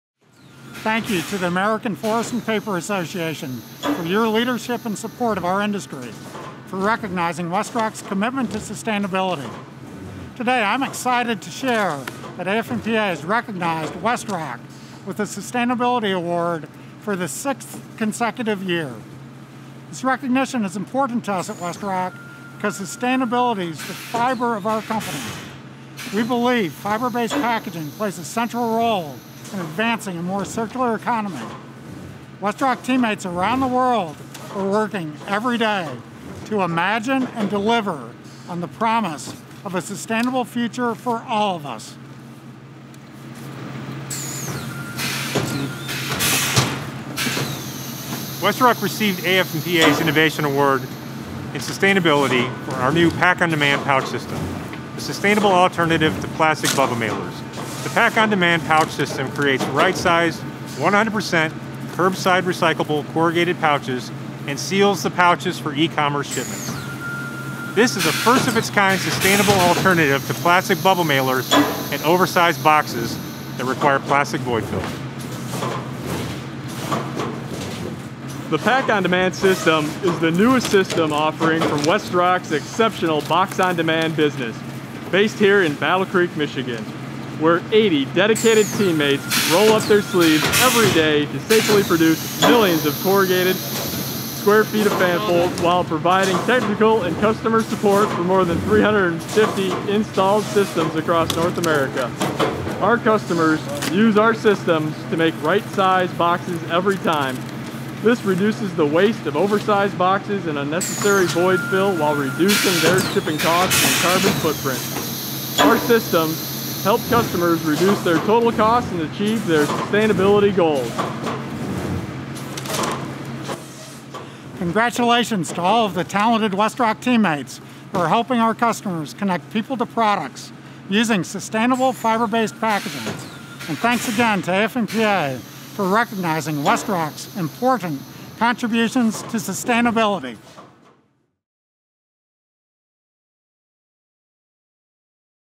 WestRock: 2020 Innovation in Sustainability Award Winner
WestRock is one of the AF&PA 2020 Innovation in Sustainability Award Winners for their project, "Pak On Demand™ Pouch System." Pak On Demand™ is a major advance for the paper-based packaging market, enabling companies to produce right-sized, highly, sustainable fiber-based pouches. The innovative pouch system uses automation to increase packing rates by up to five times. In addition, the fiber-based pouches are 100 percent curbside recyclable.